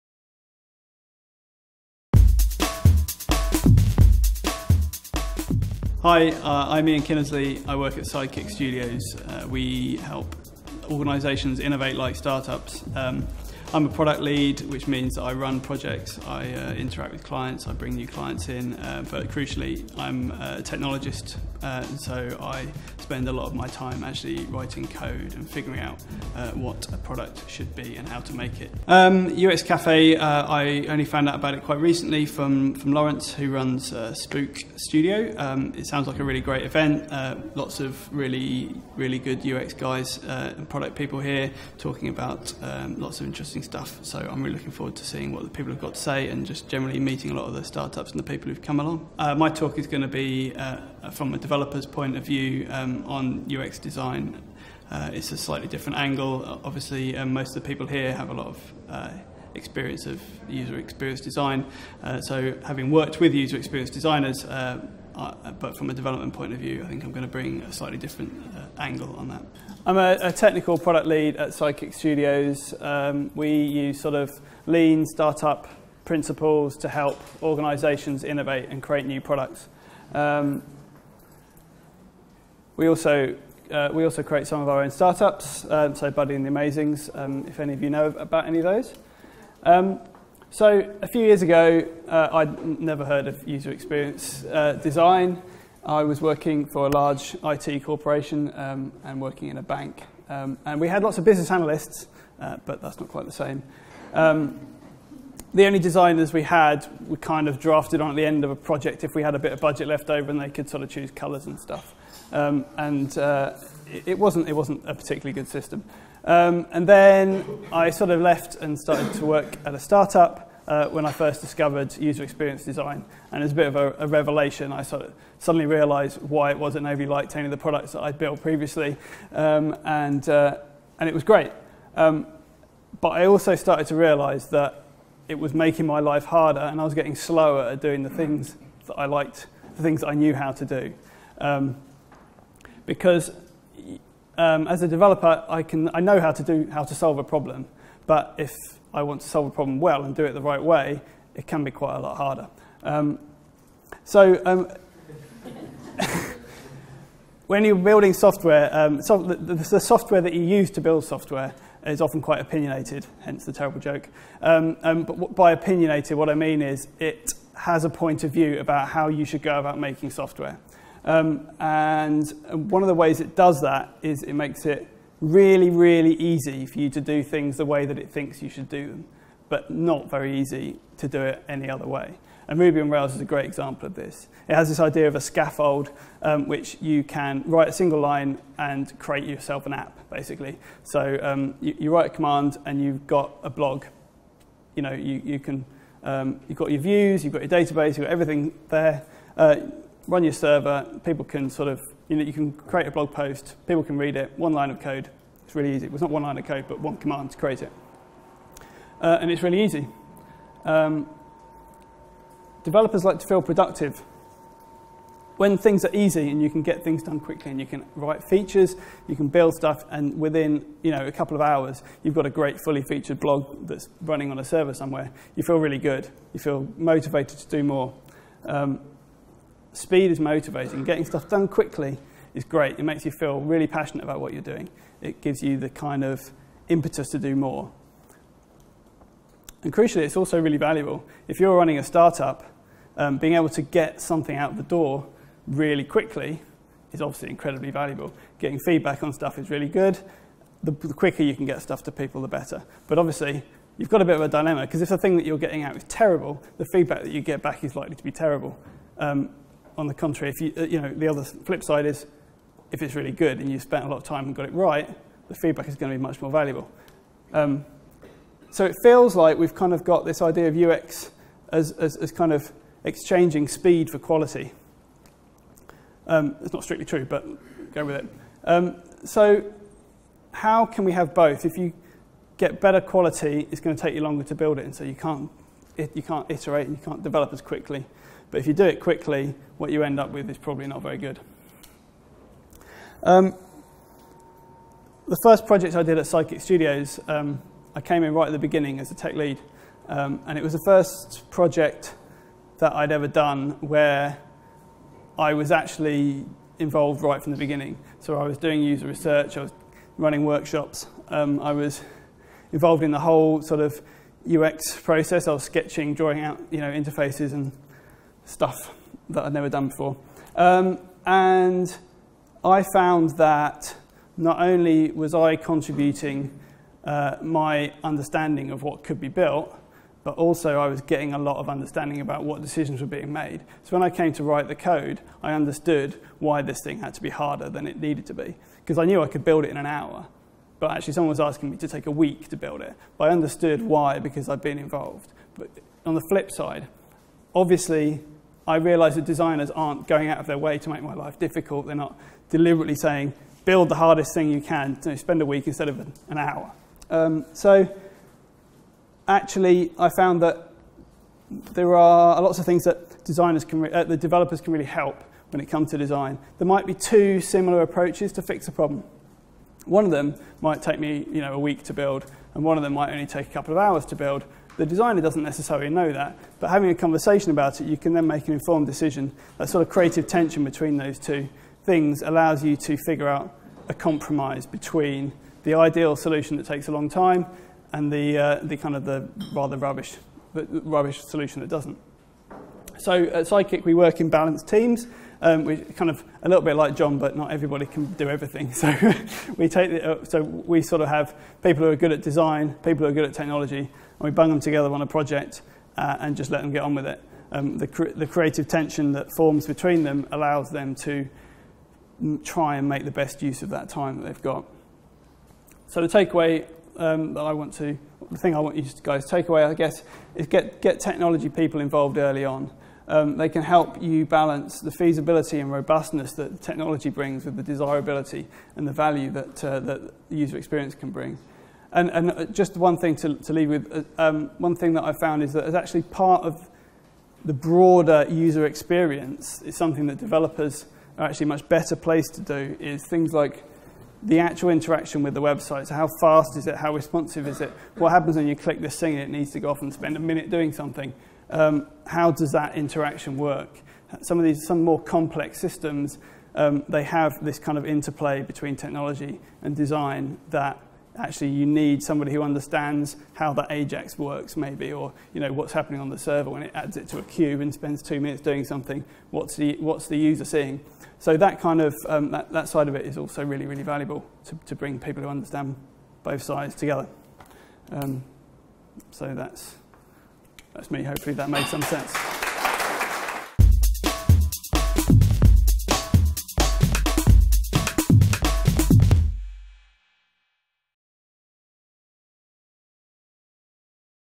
UX Café: UX for developers by Ian Kynnersley 24/7/13
Ian Kynnersley is a Product Lead from Sidekick Studios. They help companies work more like startups through innovation. In this talk Ian discusses UX design from a developers point of view.

Massive thanks to George and the team at Think Tall Films for their awesome coverage of the event.